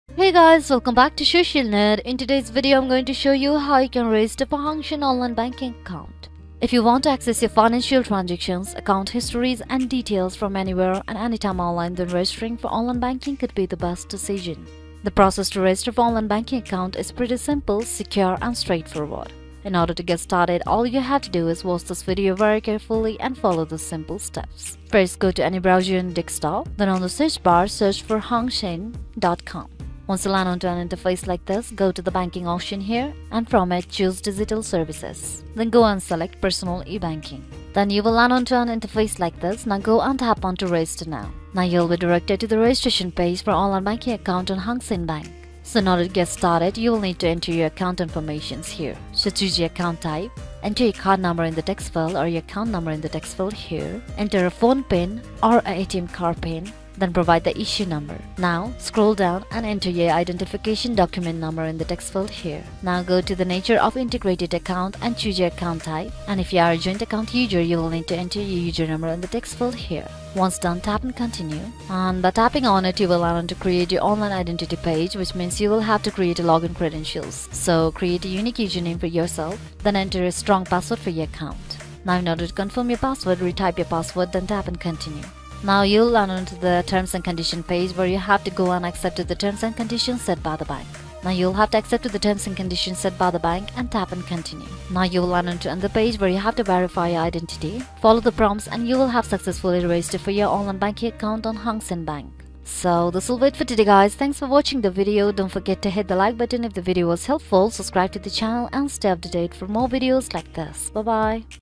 Hang Seng Bank - Register For Online Banking | Sign Up / Enroll hangseng.com
This video depicts step by step procedure to register for Hang Seng Online Banking Account. The process is pretty simple and straightforward. Just follow the instructions given below :

Go to any browser and search hangseng.com
Tap on Banking and Choose Digital Service then Select Personal eBanking
Tap on Register Now
Enter your Account Information
Create Login Credentials
Follow the prompts

And you'll have successfully registered for Hang Seng Online Banking account. Now you can have access to your account information and financial transactions from anywhere and anytime online.